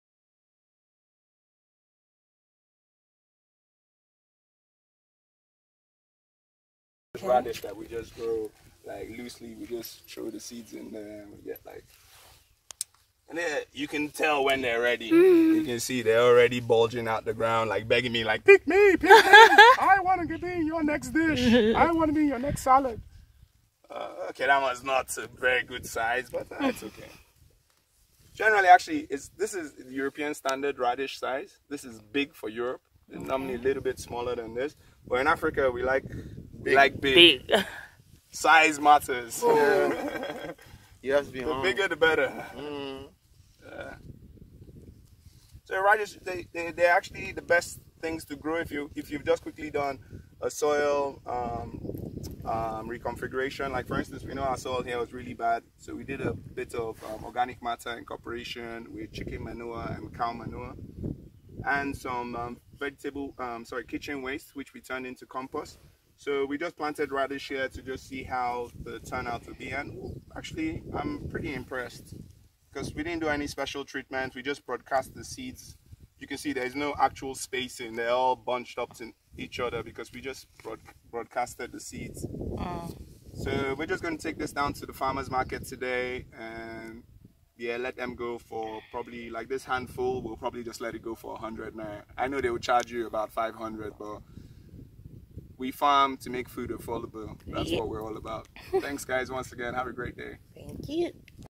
Using Radish to test soil fertility
Hi Everyone,
This is a quick video to show how to test your soil fertility using radish as they grow very fast. These were picked 30 days after planting.